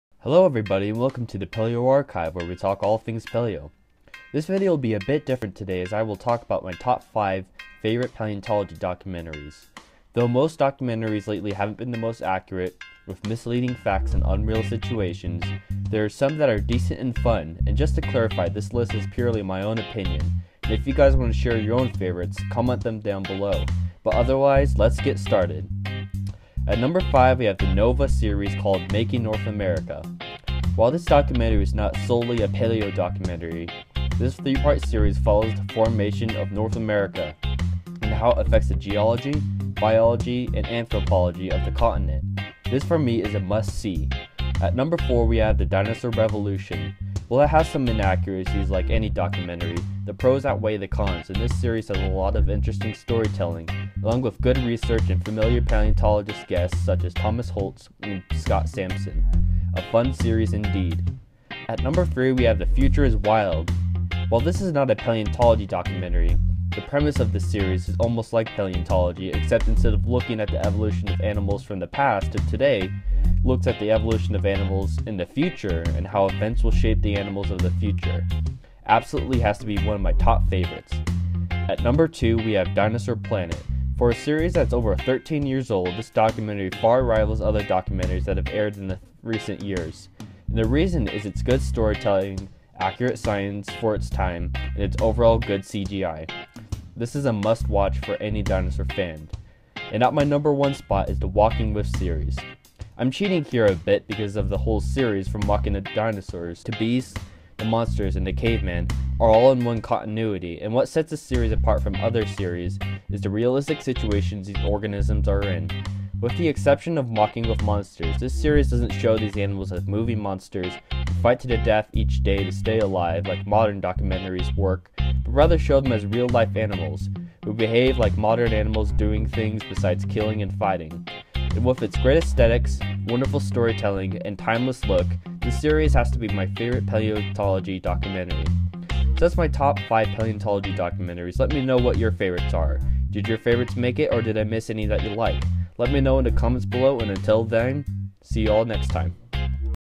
My Top 5 Favorite Paleontology Documentaries
Today we will be looking at My Top 5 Favorite Paleontology Documentaries! Note This list Is my personal list, not necessarily reflect other documentaries.